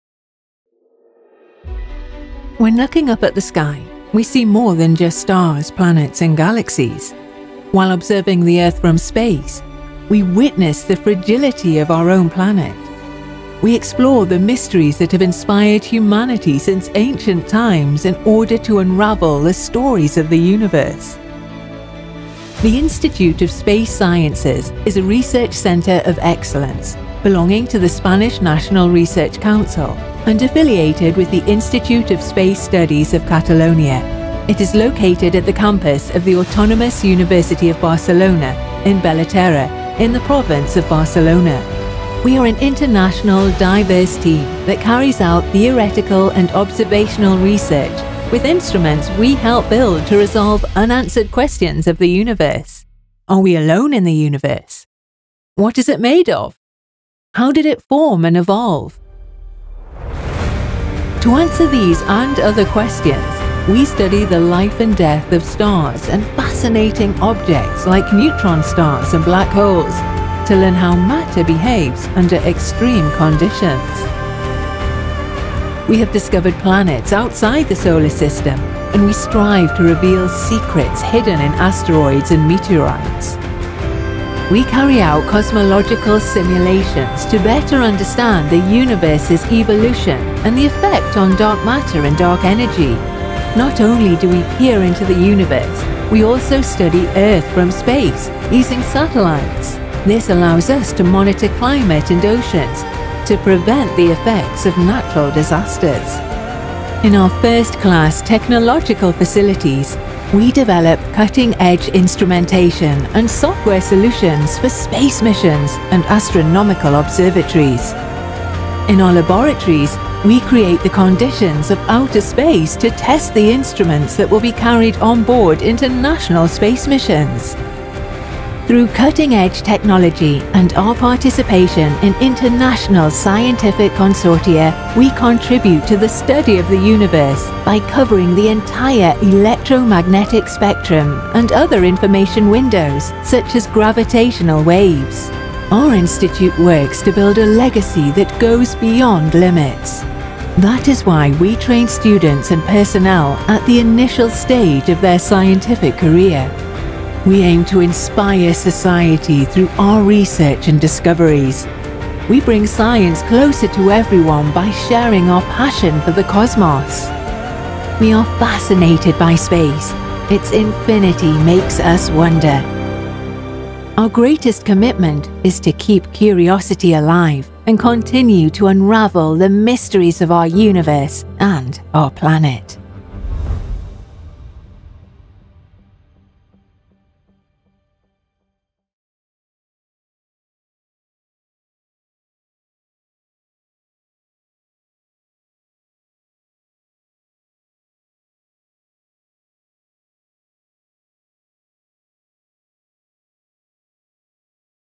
What is the Institute of Space Sciences (ICE-CSIC)?
The Institute of Space Sciences (ICE-CSIC) is a research centre of excellence that belongs to the Spanish National Research Council (CSIC) and is affiliated with the Institute of Space Studies of Catalonia (IEEC).

With an international and diverse team, ICE-CSIC carries out theoretical and observational research with instruments we help build to resolve unanswered questions about the cosmos: Are we alone in the Universe? What is it made of? How did it form and evolve?

We are fascinated by space. Let’s explore it together.